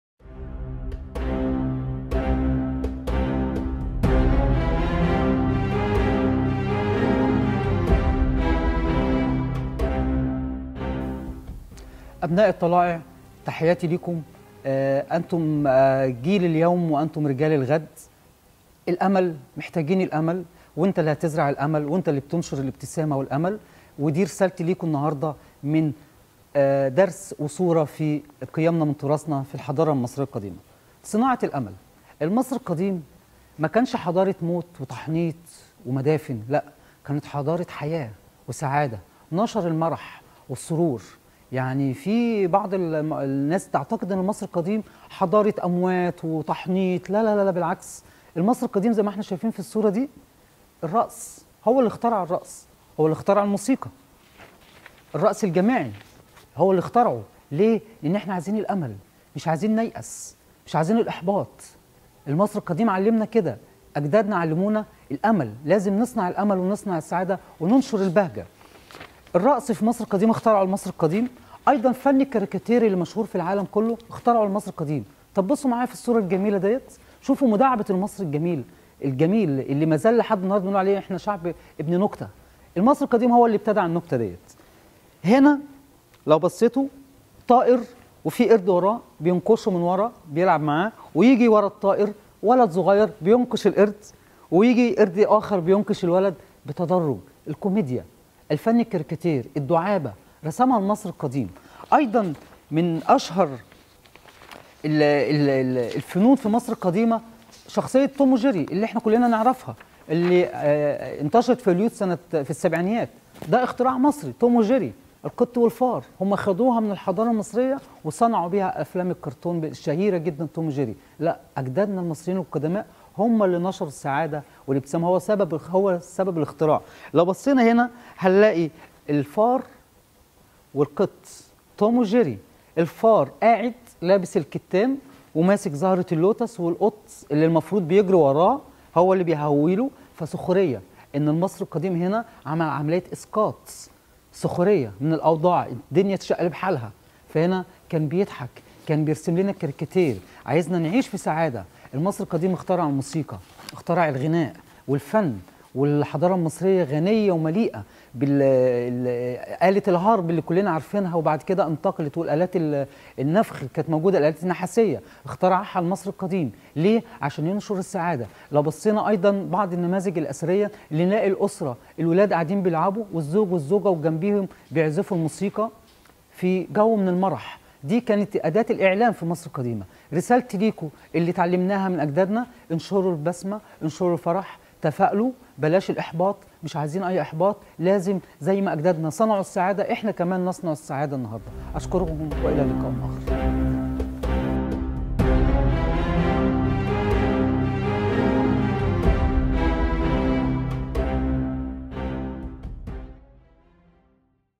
وزارة الشباب والرياضة |قيمنا من تراثنا - د عماد مهدي - صناعة الأمل
تحت رعاية الأستاذ الدكتور أشرف صبحي وزير الشباب والرياضة
 كما وعدناكم بتقديم محتوى متميز أون لاين
الدكتور / عماد مهدي عضو اتحاد الأثريين  يناقش عدد من الموضوعات مع طلائع مصر